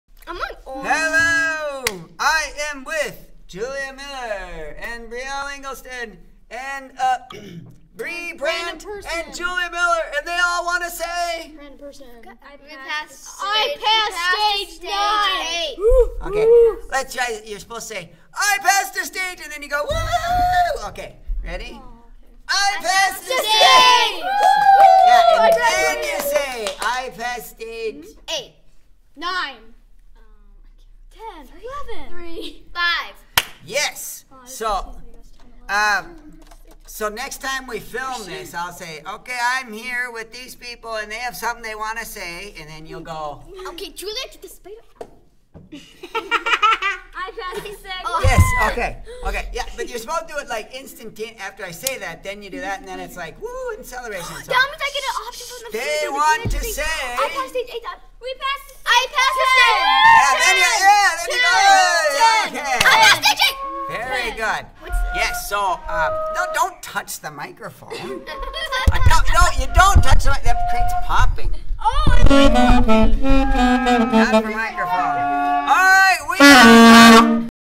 I Passed A Stage! Bree Brandt, Brielle Engelstad, Julia Miller, and Emily Johnson
Bree Brandt passed Stage 8
Brielle Engelstad passed Stage 3
Julia Miller passed Stage 8
Emily Johnson passed Stage 5